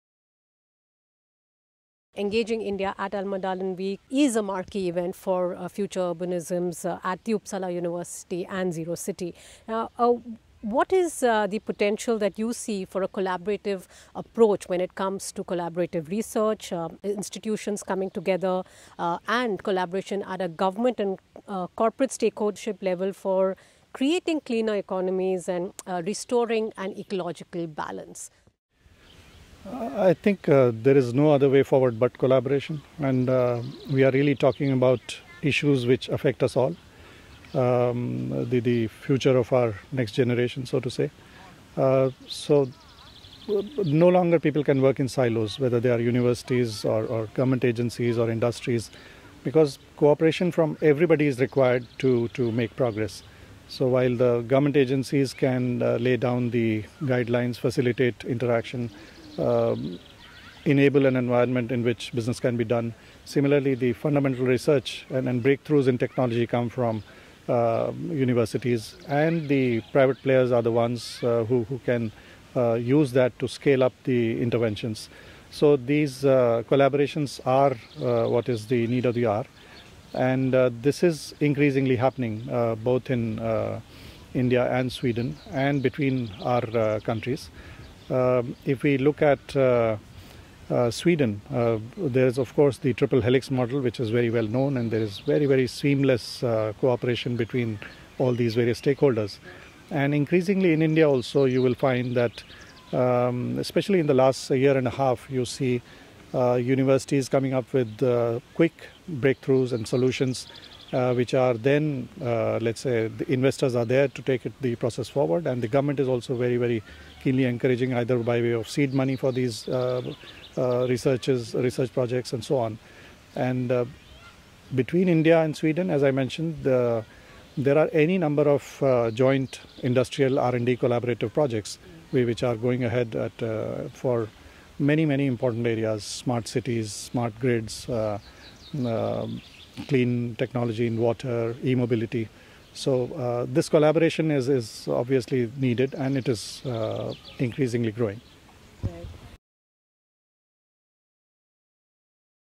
India's Ambassador to Sweden speaks on the triple helix approach at Engaging India at Almedalen
At Engaging India at Almedalen 2021: Future Urbanisms, India's Ambassador to Sweden Mr Tanmaya Lal speaks to Rupali Mehra of Content People on how a collaborative approach involving researchers, academic institutions, government and corporate stakeholders can help create cleaner economies.

Engaging India at Almedalen 2021: Future Urbanisms is organised by Future Urbanisms at Uppsala University Campus Gotland in collaboration with Content People AB and Zerocity.